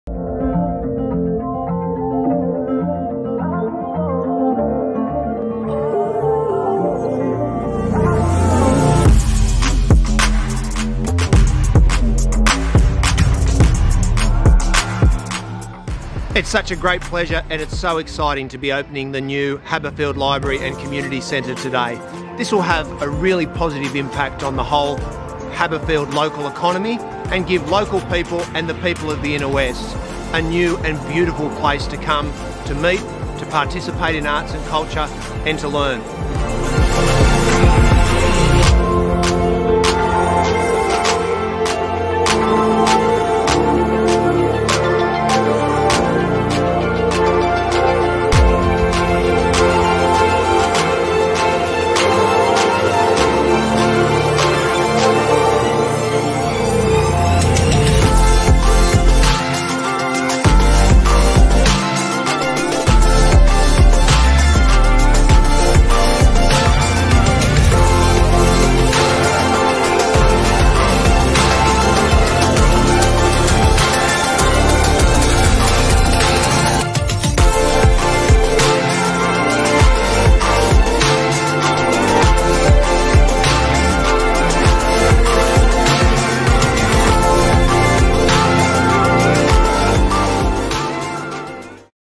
Refurbished Haberfield Library and Centre Reopening - 16 May 2021
The refurbished Haberfield Centre and Library was opened today - a beautiful facility for the Haberfield community, local economy, arts, culture and learning.
Officially opened by Darcy Byrne, Councillor - Mayor of Inner West Council, Sydney.

I 💜 Haberfield

Small Business Marketing & Community Services, go to:
or tag or message on social.